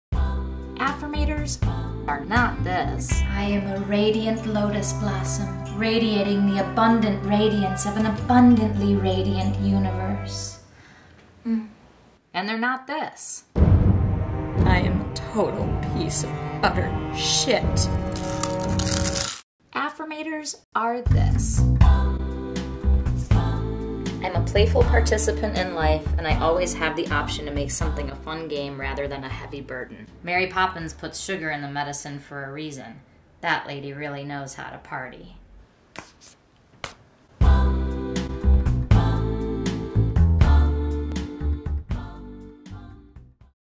Affirmators! Affirmation Cards by Suzi Barrett
Even pessimists like a little positivity, so comedian Suzi Barrett created Affirmators!—inspirational-but-not-cheesy affirmation cards. Providing the fun of a horoscope with a little less woo-woo, and full of positive affirmations, this whimsical box of 50 cards will help you help yourself, no shamanistic consult required!

Put the fun in functional with knockknockstuff.

CREDITS
Video Director: Megan Brotherton
Video Editor/Videographer: Kevin Schlanser
Starring: Suzi Barrett